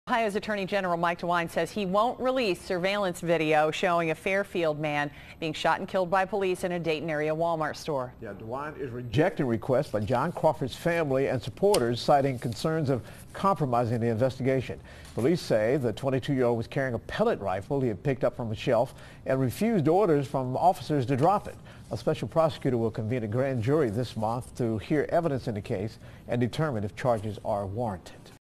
DeWine will not release Wal-Mart surveillance vi
BEAVERCREEK, Ohio (WKRC) -- Ohio attorney general Mike DeWine said he will not release surveillance video to the public showing a Fairfield man being shot and killed by police in a Dayton area Wal-Mart.

DeWine is rejecting the requests by John Crawford's family and supporters, citing concerns of compromising the investigation.

Police say the 22-year-old was carrying a pellet rifle he had picked up from a shelf and refused orders from officers to drop it.

A special prosecutor will convene a grand jury later this month to hear evidence in the case and determine if charges are warranted.